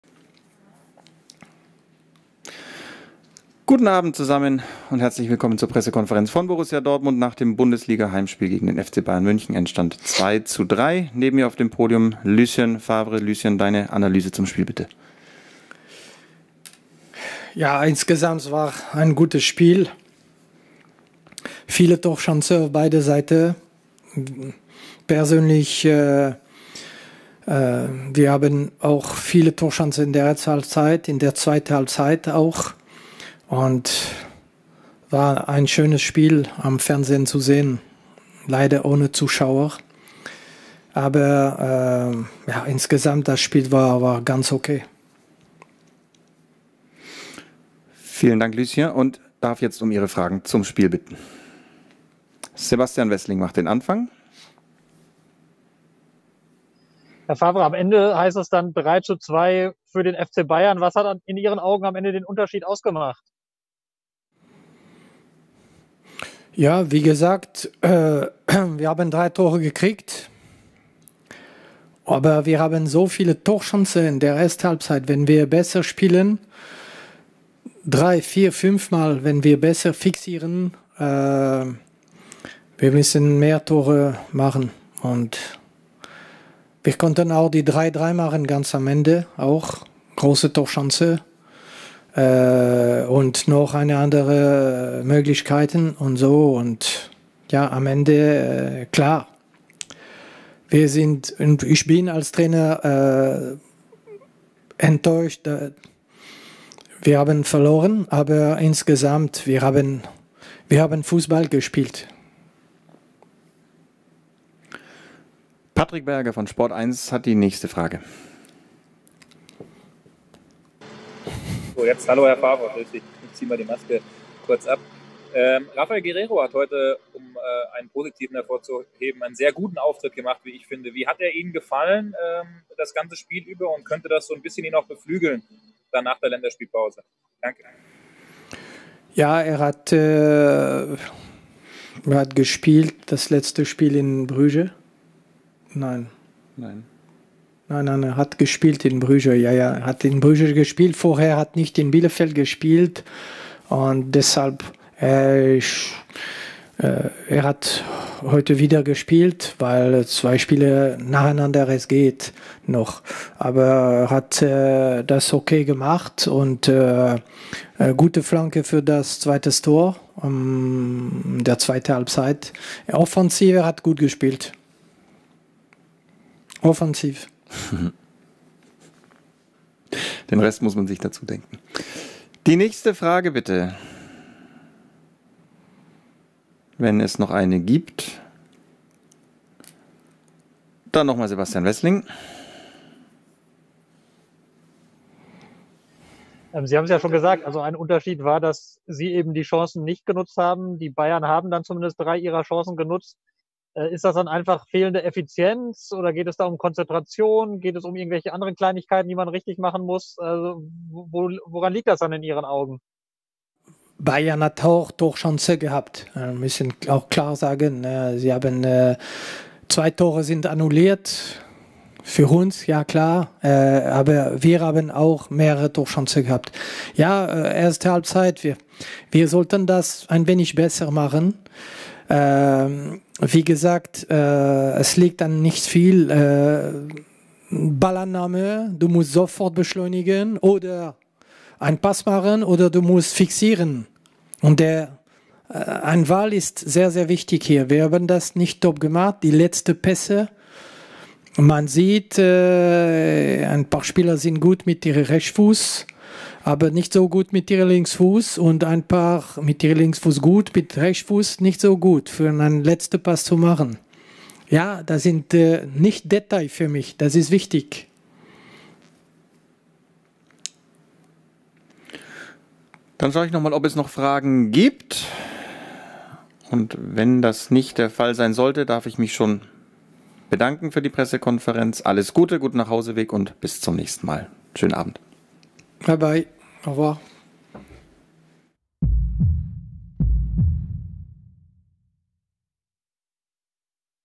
"Wir hatten viele Torchancen" | PK mit Lucien Favre | BVB – FC Bayern München
Nach wettbewerbsübergreifend vier Siegen in Serie verlor der BVB das Topspiel in der Bundesliga gegen den FC Bayern. "Wir hatten viele Torchancen, gerade in der ersten Halbzeit", resümiert BVB-Trainer Lucien Favre und attestiert seiner Mannschaft eine gute Leistung. Die gesamte Pressekonferenz seht ihr im Video!